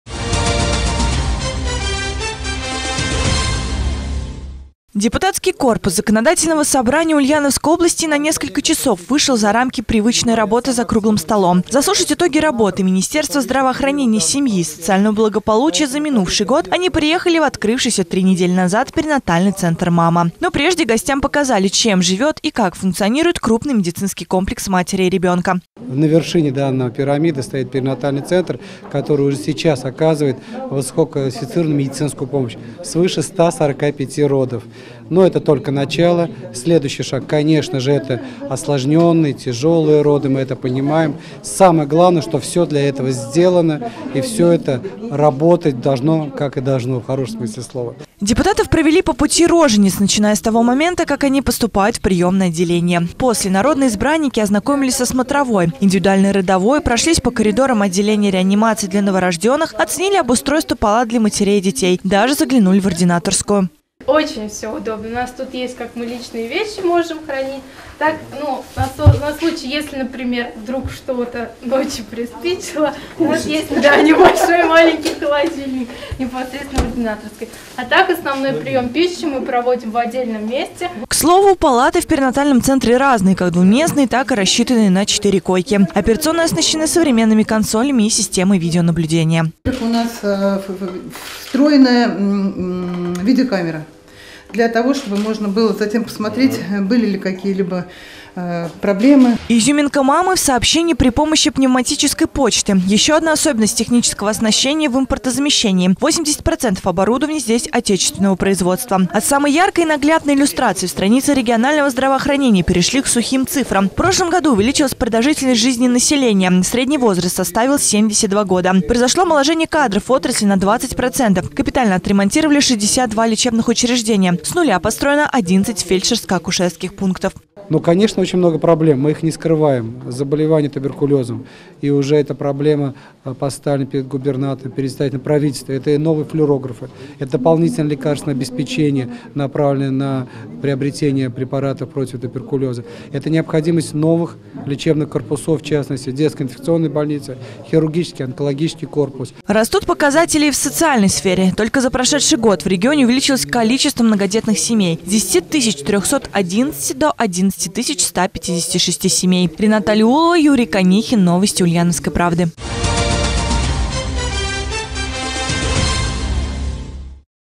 Депутаты ЗСО Ульяновской области прошли путем рожениц
Комитет по социальной политике Законодательного собрания заслушал доклад министра здравоохранения, семьи и социального благополучия Рашида Абдуллова в перинатальном центре «МАМА». Совещанию предшествовала обширная экскурсия по новому лечебному учреждению.